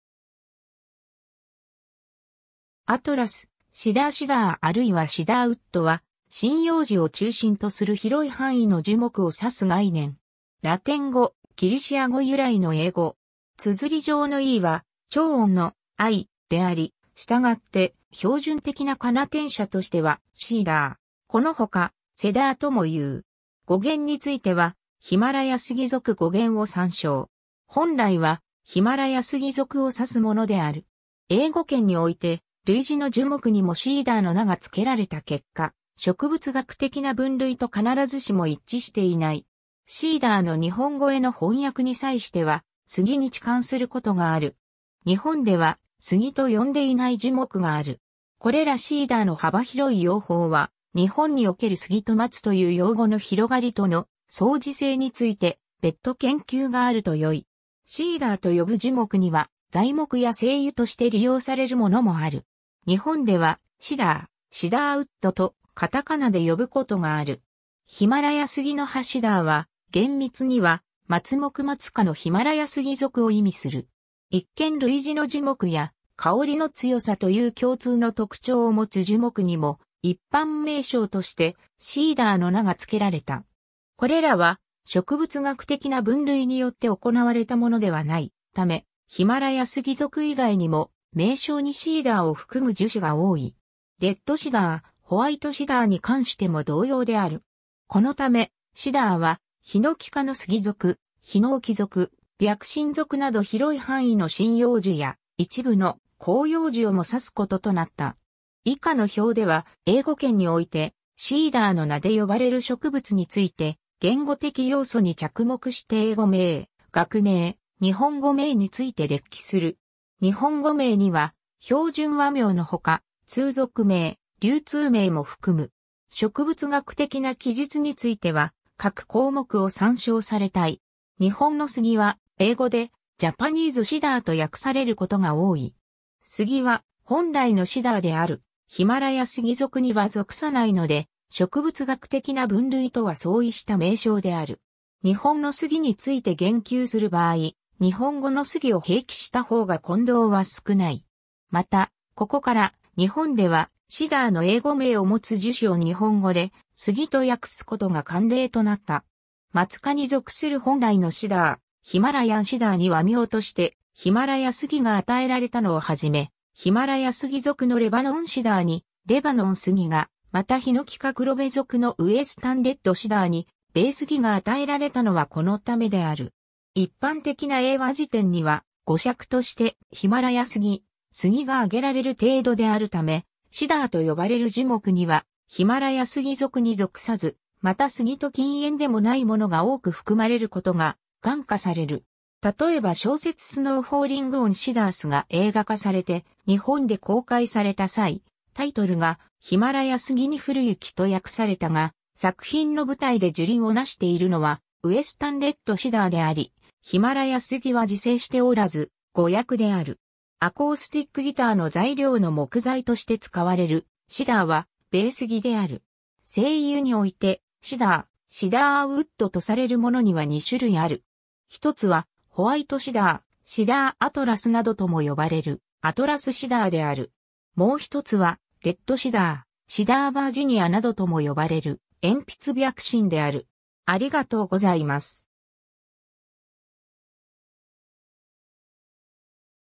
シダー
Atlas Cedar（アトラスシダー、マツ科ヒマラヤスギ属） シダー（英語: cedar） あるいは シダーウッド（英語: cedar wood）は、針葉樹を中心とする広い範囲の樹木を指す概念。
ラテン語・ギリシア語由来の英語。
綴り上のeは長音の/iː/であり、したがって標準的な仮名転写としては「シーダー」。
このほか、「セダー」ともいう。
語源についてはヒマラヤスギ属#語源を参照。
 本来はヒマラヤスギ属（マツ科）を指すものである。
英語圏において類似の樹木にも cedar の名が付けられた結果、植物学的な分類と必ずしも一致していない。
 cedar の日本語への翻訳に際しては「杉(スギ)」に置換することがある。
日本ではスギと呼んでいない樹木がある。
これら cedar の幅広い用法は、日本における杉と松という用語の広がりとの相似性について別途研究があるとよい。
 cedar と呼ぶ樹木には材木や精油（エッセンシャルオイル）として利用されるものもある。
日本では「シダー」「シダーウッド」とカタカナで呼ぶことがある。
 ヒマラヤスギの葉 Cedar は、厳密にはマツ目マツ科のヒマラヤスギ属 (Cedrus) を意味する。
一見類似の樹木や、「香りの強さ」という共通の特徴を持つ樹木にも一般名称として cedar の名が付けられた。
これらは植物学的な分類によって行われたものではないため、ヒマラヤスギ属以外にも、名称に cedar を含む樹種が多い。
レッドシダー、ホワイトシダーに関しても同様である。
このため「シダー」は、ヒノキ科のスギ属・ヒノキ属・ビャクシン属など広い範囲の針葉樹や、一部の広葉樹をも指すこととなった。
 以下の表では、英語圏において cedar の名で呼ばれる植物について、言語的要素に着目して英語名・学名・日本語名について列記する。
「日本語名」には標準和名のほか、通俗名・流通名も含む。
植物学的な記述については各項目を参照されたい。
 日本のスギ（杉,Cryptomeria japonica）は、英語で Japanese Cedar と訳されることが多い。
スギ（ヒノキ科スギ属もしくはスギ科）は本来の Cedar であるヒマラヤスギ属には属さないので、植物学的な分類とは相違した名称である。
日本のスギについて言及する場合、日本語の Sugi を併記した方が混同は少ない。
 またここから、日本では Cedar の英語名を持つ樹種を日本語で「スギ」と訳すことが慣例となった。
マツ科に属する本来の Cedar (Himalayan Cedar）に和名として「ヒマラヤスギ」が与えられたのをはじめ、 ヒマラヤスギ属の Lebanon Cedar に「レバノンスギ」が、またヒノキ科クロベ属の Western Red Cedar に「ベイスギ」が与えられたのはこのためである。
 一般的な英和辞典には語釈として「ヒマラヤスギ」「スギ」が挙げられる程度であるため、 Cedar と呼ばれる樹木にはヒマラヤスギ属に属さず、またスギと近縁でもないものが多く含まれることが看過される。
たとえば小説 Snow Falling on Cedars が映画化されて日本で公開された際、タイトルが『ヒマラヤ杉に降る雪』と訳されたが、作品の舞台（アメリカ西北部）で樹林をなしているのは Western Red Cedar（ベイスギ）であり、ヒマラヤスギは自生しておらず、誤訳である。
 アコースティック・ギターの材料の木材として使われる「シダー」は、ベイスギ（Western Red Cedar, Thuja plicata、ヒノキ科クロベ属）である。
 精油（エッセンシャルオイル）において「シダー」「シダーウッド」とされるものには2種類ある。
 1つは「ホワイトシダー」「シダーアトラス」などとも呼ばれるアトラスシダー（Atlas Cedar, Cedrus atlantica, マツ科ヒマラヤスギ属）である。
 もう1つは「レッドシダー」「シダーバージニア」などとも呼ばれるエンピツビャクシン（Eastern Red Cedar, Juniperus virginiana, ヒノキ科ビャクシン属）である。